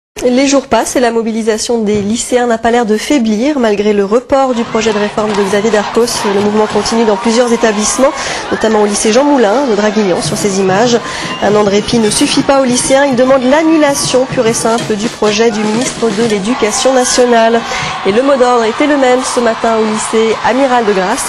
Lycée jean Moulin à draguignan contre la réforme Darcos sur FRANCE 3
Mobilisation nationale du 18/12/08 contre la réforme de X.Darcos,le Lycée Jean moulin se lève et dit NON,ce blocus est un succès et sera suivi d'une manifestation jusqu'à la sous-préfecture.